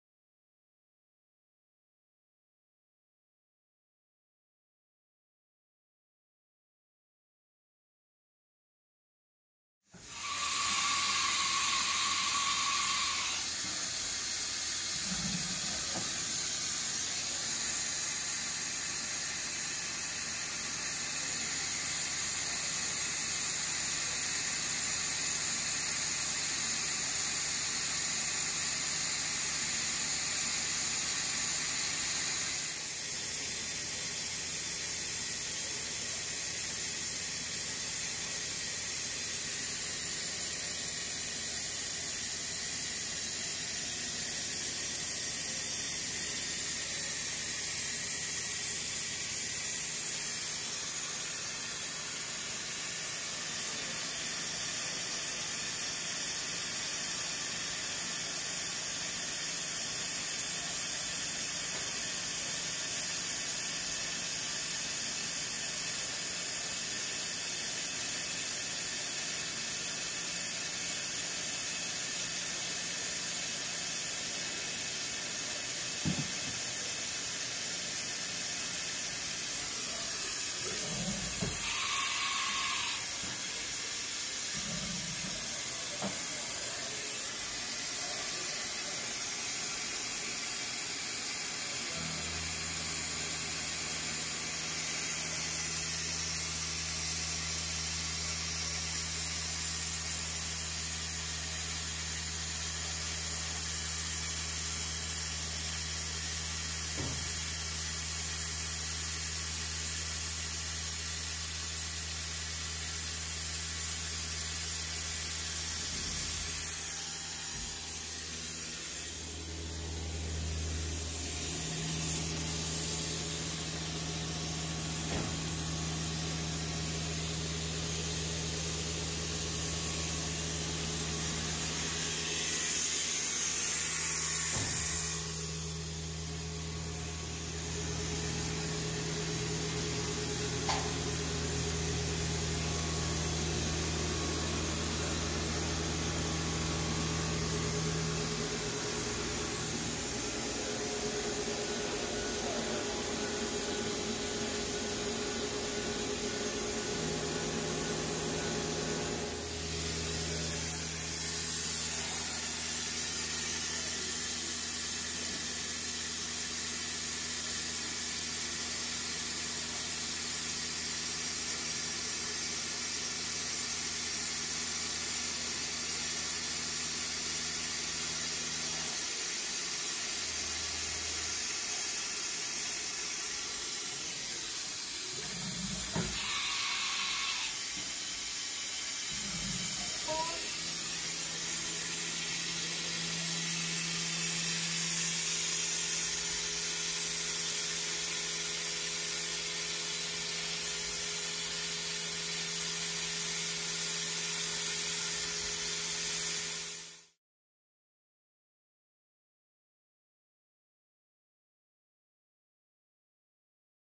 1998 Haas VF-0E CNC Vertical Mill w/4th Axis Rotary Table
This 1998 Haas VF-0E CNC Vertical Mill w/4th Axis Rotary Table - Item # 09985 is available for sale from Resell CNC.